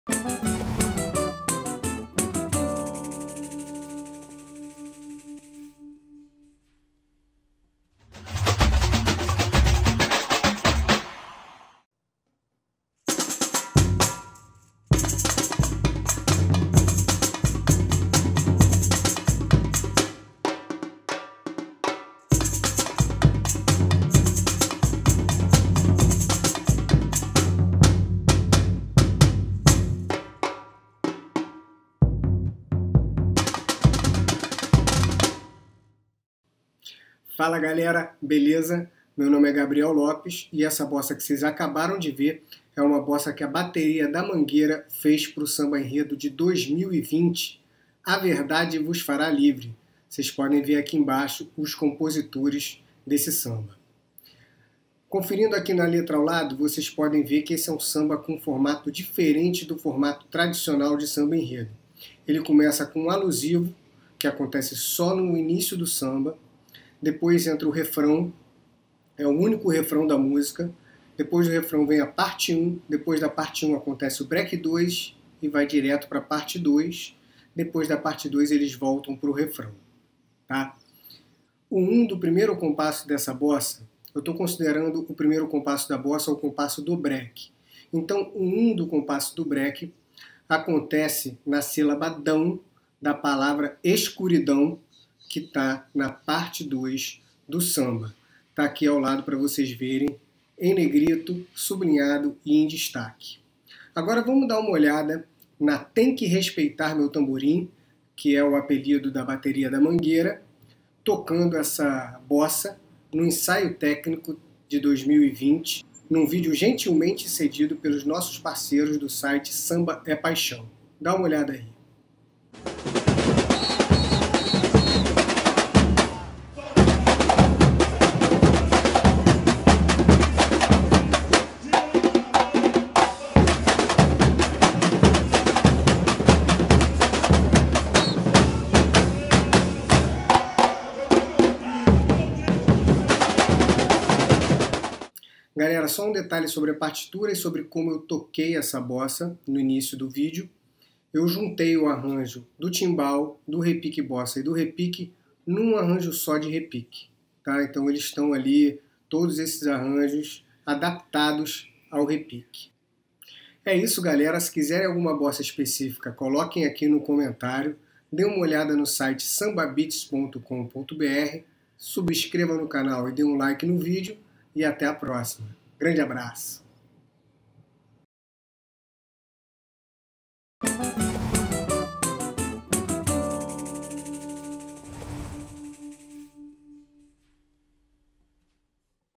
SéP | Samba Beats - Bossas - Mangueira 2020 (English Subtitles)
Nesse video falamos de uma das bossas (paradinhas) que a bateria da Mangueira fez para o Samba do carnaval de 2020.

In this video we show the arrangement of a break that the Mangueira's bateria made for 2020's carnival.

O video da bateria foi gentilmente cedido pelo canal Samba é Paixão / The video of Vila's bateria was kindly provided by Samba é Paixão channel.